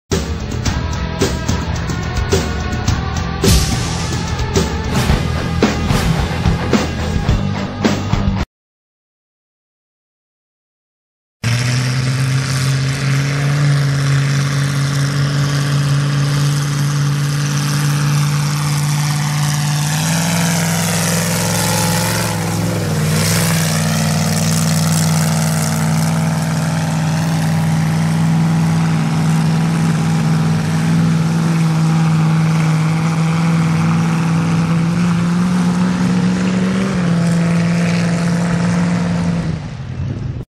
Bucking Bull Ford Mudding At Walton Mud Bog
Footage from the Walton Raceway Mud Bog May 18, 2014